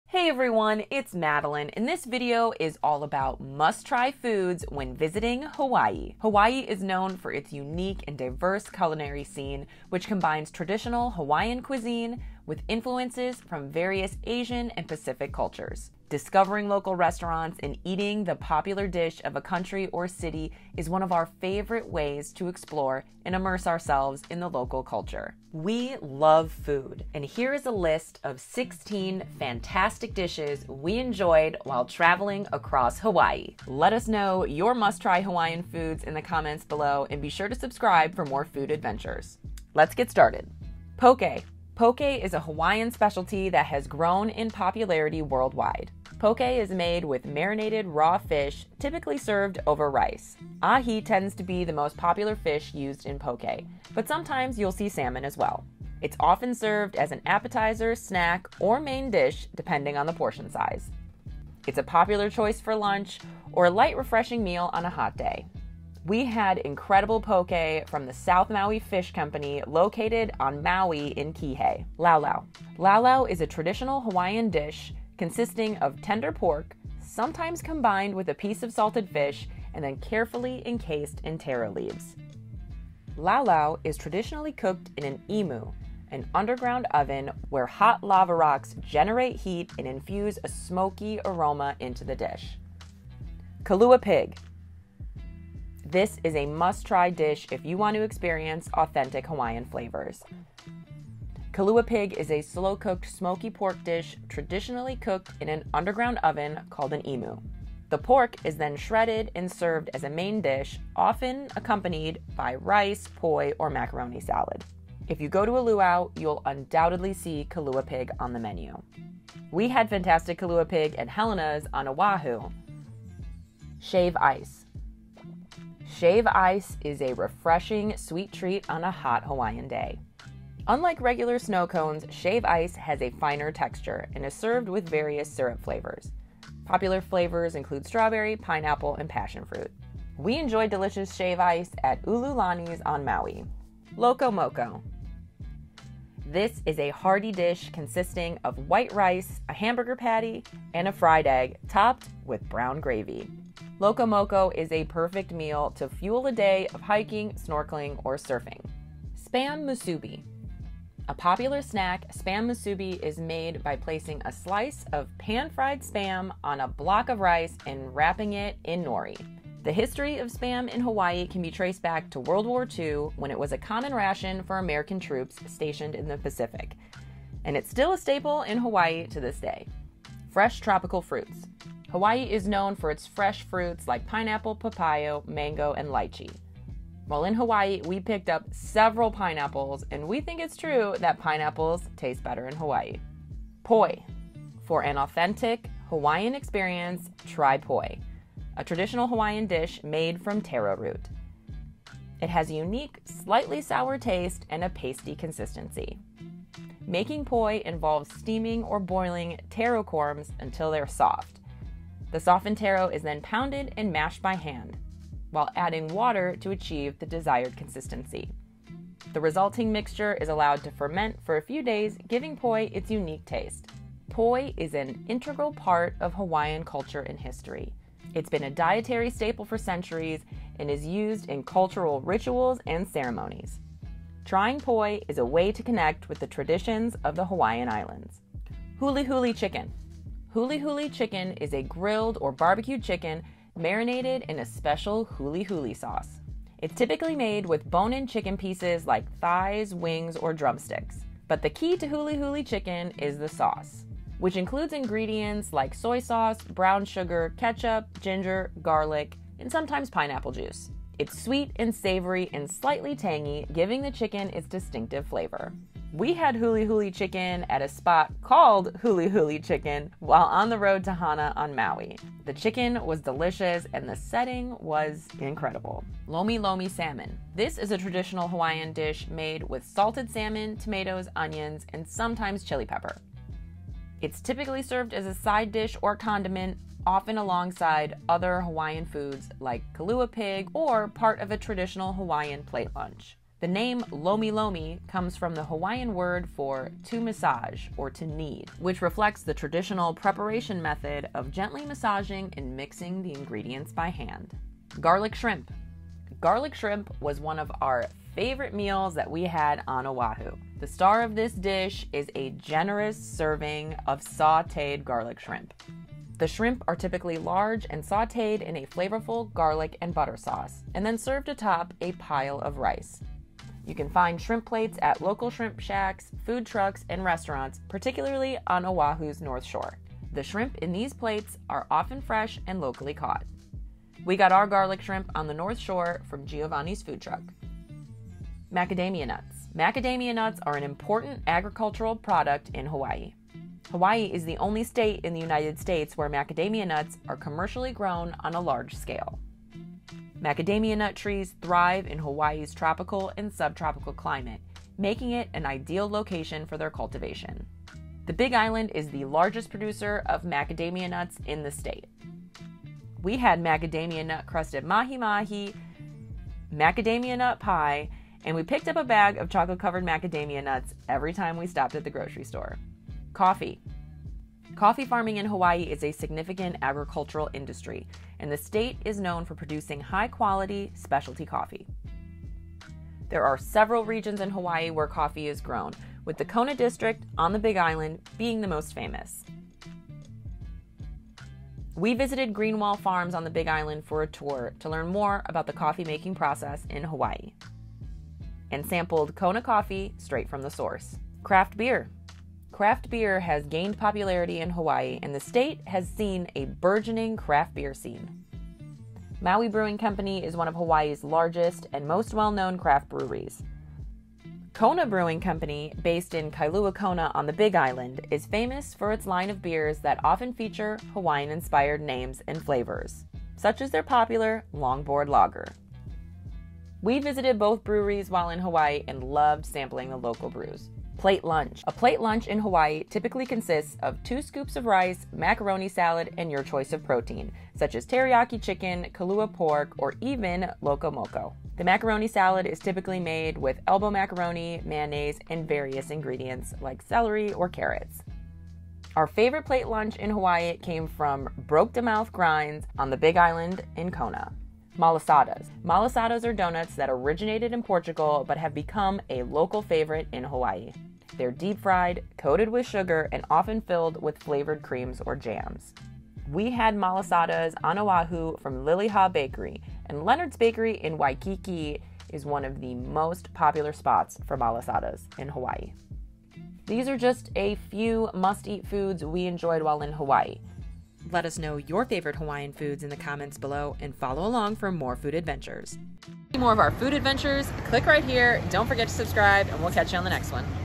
16 Must Try Foods in Hawaii | Best Food and Drinks in Hawaii
This video is all about Must Try Foods when visiting Hawaii. Hawaii is known for its unique and diverse culinary scene, which combines traditional Hawaiian cuisine with influences from various Asian and Pacific cultures. We list 16 of the best Hawaiian foods and drinks to try on your Hawaiian vacation.

00:00 Must Eat Food in Hawaii

**All Drone footage is our footage obtained legally by a Remote Pilot under the FAA 's Small UAS Rule (Part 107). All footage is obtained outside of National Parks, State Parks, and other areas where flying drones is not permitted.**